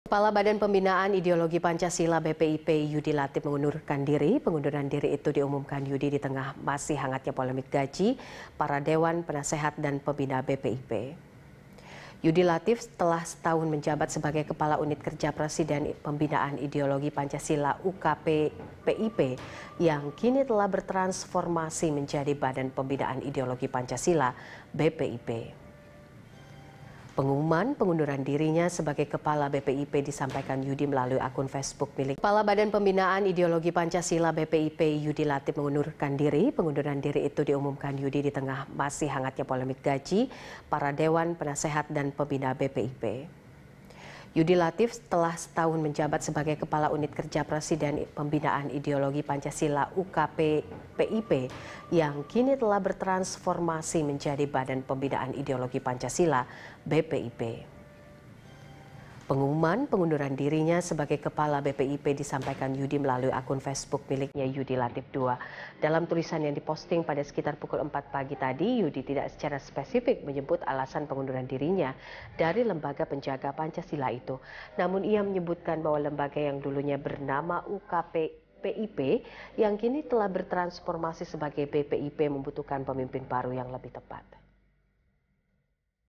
Yudi Latif Mundur sebagai Kepala BPIP
Kepala Badan Pembinaan Ideologi Pancasila (BPIP) Yudi Latif mengundurkan diri. Pengunduran diri itu diumumkan Yudi di tengah masih hangatnya polemik gaji para Dewan Penisehat dan Pembina BPIP.

Facebook.com/BeritaSatuTV
Youtube.com/BeritaSatu